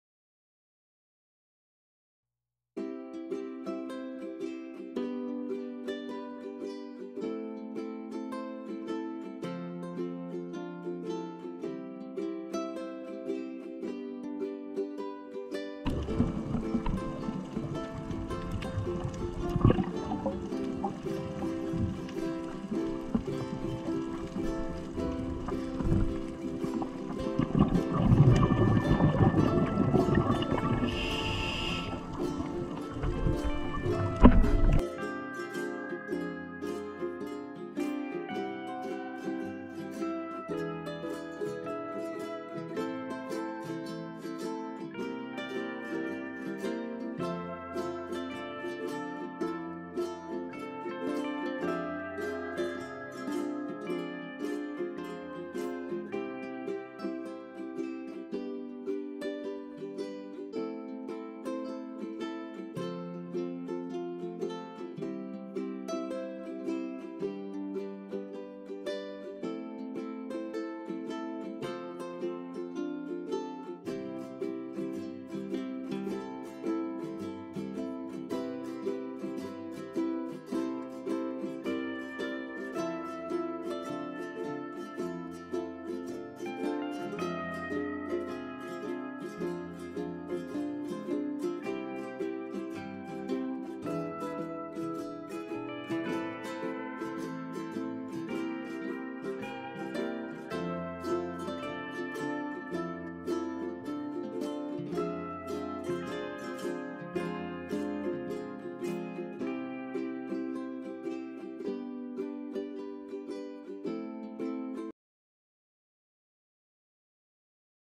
Mellow Mauna Lani (South)
An easy and longish dive off Mauna Lani. We were greeted and sent off by a juvenile Eagle Ray in the shallows.  Encountered large yellowtail moray (?) and usual suspects around a nice coral garden.